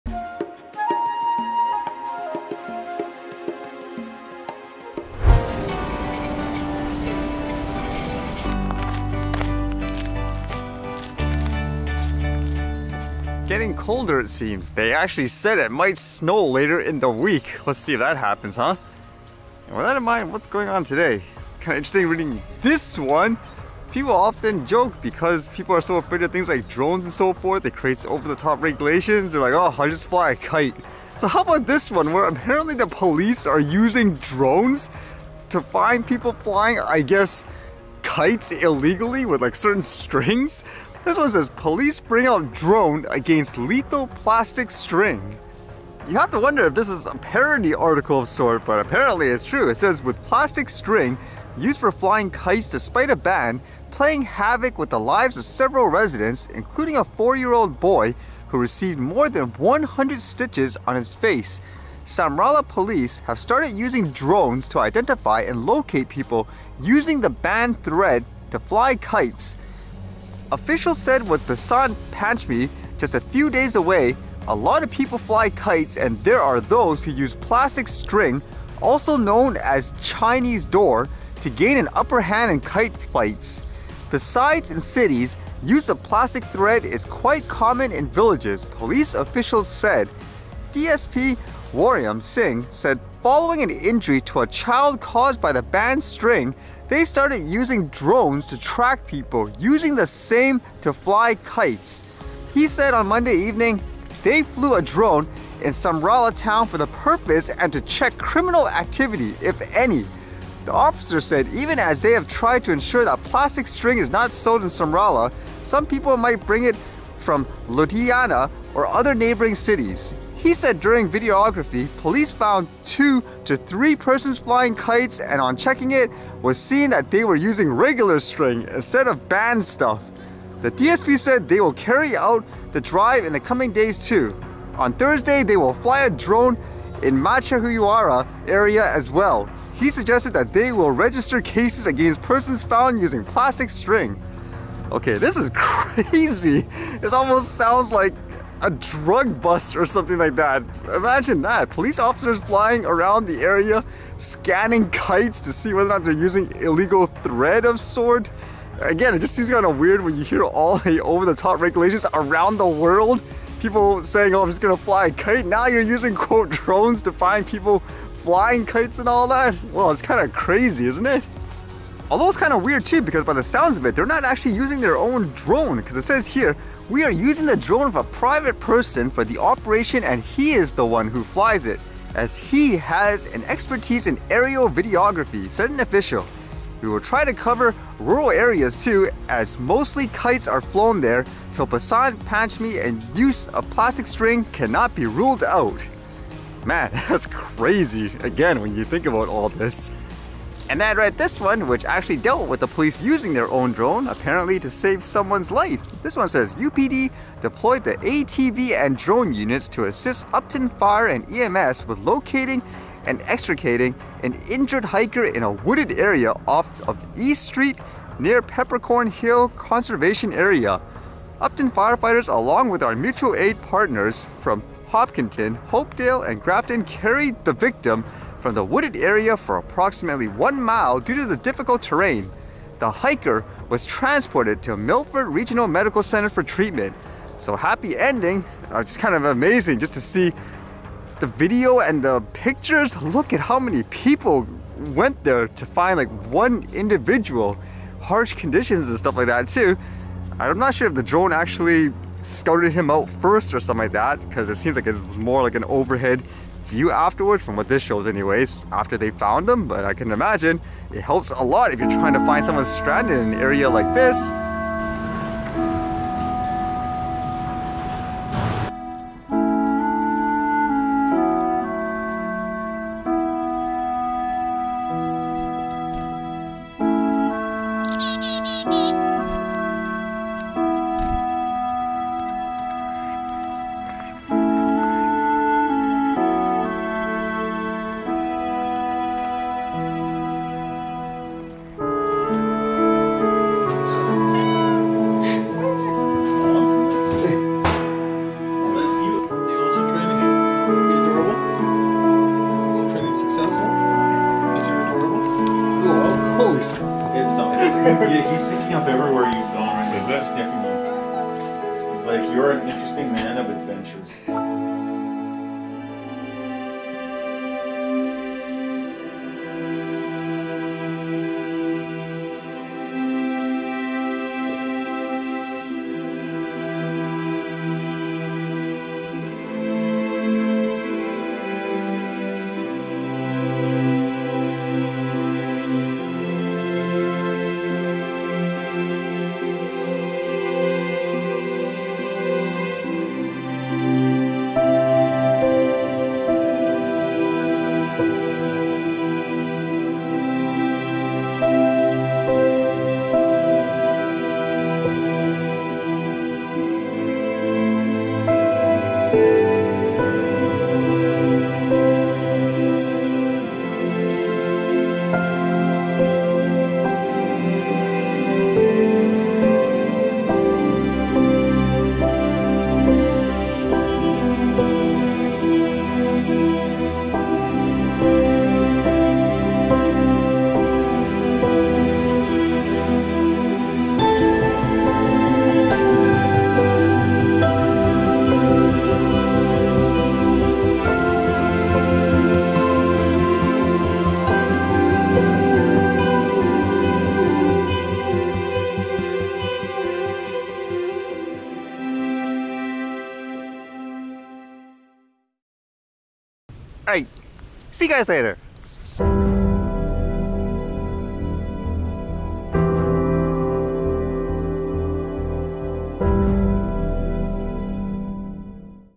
Police Drone Monitoring For Kites, Wooded Area Hiker Rescue
-Police using drones to find kite flyers using lethal plastic strings for flights
-Hiker rescued with a team using ATVs and drones
-Squirrels, birds and geese
-Thermal video of ducks
-Drone video of cloudy skies and buildings

0:00 Intro and Weather
0:19 Drones and Kites
3:20 Hiker Rescue
4:24 Wildlife And Thermal
5:15 Drone Video